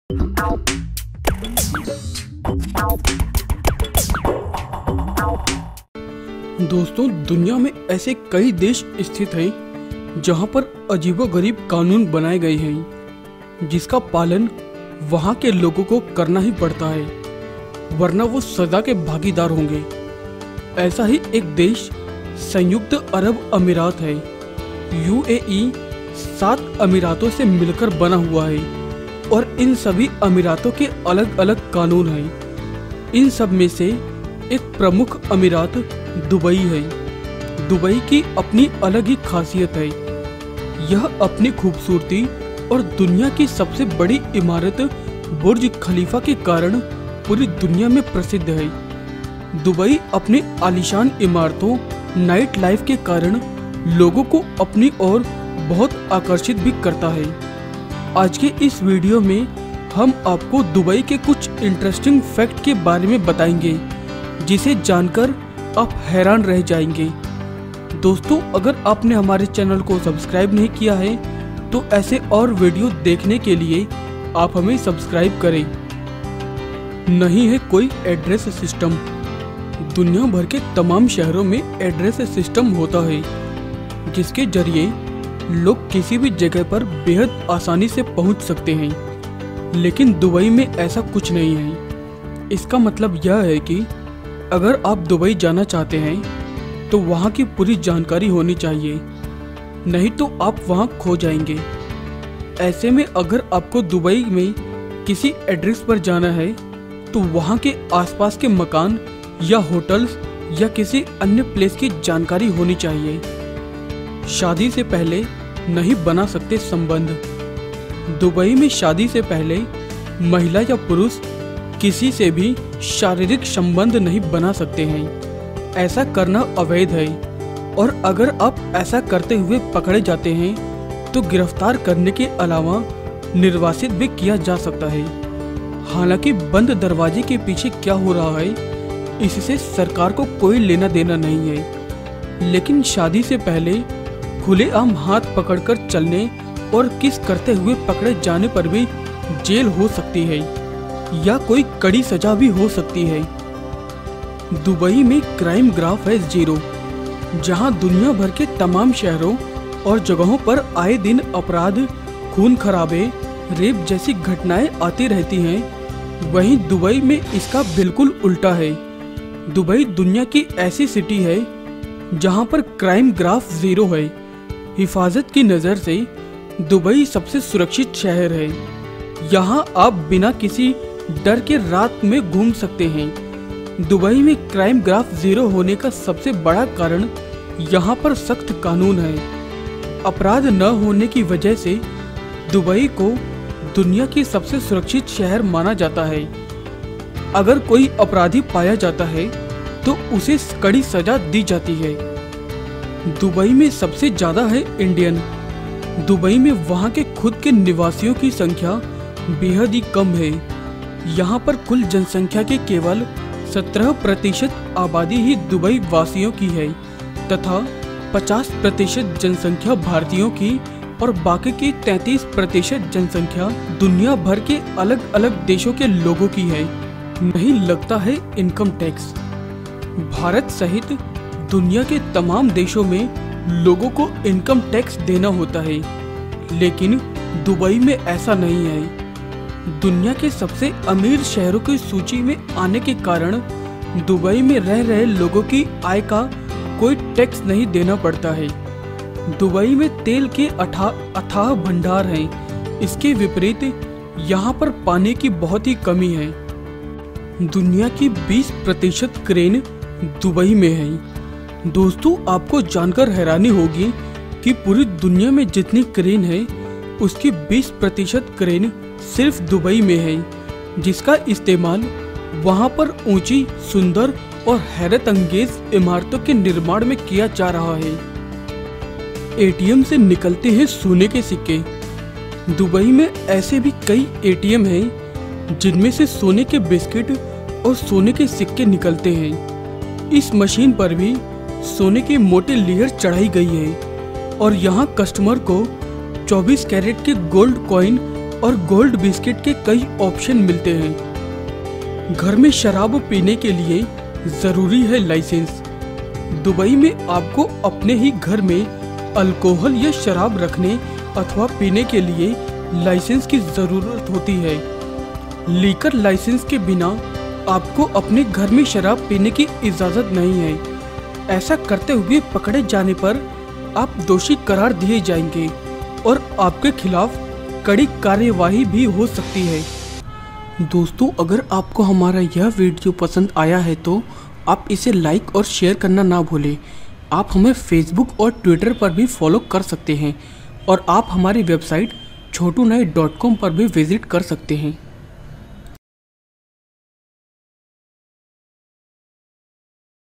जानिए दुबई के कुछ इंटरेस्टिंग फैक्ट्स
दुनिया में कई ऐसे देश स्थित हैं, जहां पर अजीबोगरीब कानून बनाए गए हैं। जिसका पालन वह के लोगों को करना पड़ता है, वरना वो सजा के भागीदार होंगे | ऐसा ही एक देश संयुक्त अरब अमीरात (UAE) है। यूएई सात अमीरातों से मिलकर बना है और इन सभी अमीरात के अलग अलग कानून हैं। इन सब में से एक प्रमुख अमीरात दुबई है। दुबई की अपनी अलग ही खासियत है | यह अपनी खूबसूरती, और दुनिया की सबसे बड़ी ईमारत बुर्ज खलीफा के कारन पूरी दुनिया में प्रसिद्ध है | दुबई अपने आलीशान इमारतों, नाटइलाइफ के कारण लोगों को अपनी ओर बहुत आकर्षित करता है। आज के इस वीडियो में हम आपको दुबई के कुछ इंट्रेस्टिंग फैक्ट के बारे में बताऊंगा जिसे जानकार आप भी हैरान हो जायेंगे |

नहीं है कोई एड्रेस सिस्टम
दुनियाभर के तमाम शहरों में एड्रेस सिस्टम होता है, जिसके जरिए लोग किसी भी जगह पर बेहद आसानी से पहुँच सकते है | लेकिन दुबई में ऐसा कुछ नहीं है। इसका मतलब यह की अगर आप दुबई जाना चाहते है तो वह की पूरी जानकारी होनी चाहिए नहीं तो आप वह खो जायेंगे | ऐसे में अगर आपको दुबई में किसी एड्रेस पर जाना है तो वहां के आस-पास के मकान या होटल्स या किसी अन्य प्लेस की जानकारी होनी चाहिए।

शादी से पहले नहीं बना सकते संबंध
दुबई में शादी से पहले महिला-पुरुष किसी से भी शारीरिक सम्बन्ध नहीं बना सकते है | ऐसा करना अवैध है। और अगर आप ऐसा करते हुए पकड़े जाते हैं तो गिरफ्तार करने के अलावा निर्वासित भी किया जा सकता है। हालांकि, बंद दरवाजे के पीछे क्या हो रहा है इससे सरकार को कोई लेना-देना नहीं है। लेकिन शादी से पहले खुलेआम हाथ पकड़ कर चलने और किस करते हुए पकड़े जाने पर भी जेल हो सकती है। यह कोई कड़ी सजा भी हो सकती है |

दुबई में क्राइम ग्राफ है जीरो
जहा दुनियाभर के तमाम शहरो और जगहों पर आये दिन अपराध, खून खराब, रेप जैसी घटनाये आती रहती है | वही दुबई में इसका बिलकुल उल्टा है | दुबई दुनिया की ऐसी सिटी है जहा पर क्राइम ग्राफ जीरो है | हिफाजत की नज़र से दुबई सबसे सुरक्षित शहर है | यहाँ आप बिना किसी डर के साथ रात को भी घूम सकते है | दुबई में क्राइम ग्राफ जीरो होने का सबसे बड़ा कारन यहाँ पर सख्त कानून है |अपराध न होने की वजह से ही दुबई को दुनिया का सबसे सुरक्षित शहर माना जाता है | अगर कोई अपराधी पाया जाता है तो उसे कड़ी सजा दी जाती है |

दुबई में सबसे ज्यादा है इंडियन
दुबई में वहा के खुद के निवासियों की संख्या बहुत कम है | यहाँ पर कुल जनसँख्या की केवल 17 प्रतिशत आबादी ही दुबईवासियों की तथा 50 प्रतिशत जनसंख्या भारतीयों की है। बाकी की 33 प्रतिशत जनसंख्या दुनियाभर के अलग-अलग देशों के लोगों की है।

नहीं लगता है इनकम टैक्स
भारत सहित दुनियाभर के तमाम देशों में लोगों को इनकम टैक्स देना होता है, लेकिन दुबई में ऐसा नहीं है। दुनिया के सबसे आमिर शहरो की सूचि में आने के कारन दुबई में रह रहे लोगो को अपनी आय का कोई टैक्सी नहीं देना पड़ता है | दुबई में तेल के अथाह भंडार है इसके विपरीत यहाँ पर पानी की कमी है

दुनिया की 20 प्रतिशत क्रेन दुबई में
दोस्तों आपको जानकर हैरानी होगी की पूरी दुनिया में जितनी क्रेन है उसकी 20 प्रतिशत क्रेन सिर्फ दुबई में है | जिसका इस्तेमाल वहाँ पर ऊँची, सूंदर और हैरतअंगेज इमारतों के निर्माण में किया जा रहा है |

एटीएम से निकलते है सोने के सिक्के
दुबई में ऐसे भी कई एटीएम हैं, जिसमें से सोने के बिस्किट और सोने के सिक्के निकलते हैं। इस मशीन पर भी सोने की मोटी लेयर चढ़ी होती है और यहां कस्टमर्स को 24 कैरेट के गोल्ड क्वाइन और गोल्ड बिस्किट के कई ऑप्शन मिलते हैं।

घर में शराब पीने के लिए भी जरुरी है लाइसेंस
दुबई में आपको अपने ही घर में एल्कोहल या शराब रखने के लिए लाइसेंस जरूरी होता है। लिकर लाइसेंस के बिना आपको अपने घर में शराब पीने की इजाजत नहीं होती है | ऐसा करते हुए पकडे जाने पर आप दोषी करार दिए जायेंगे और आपके खिलाफ कड़ी कार्यवाही भी हो सकती है |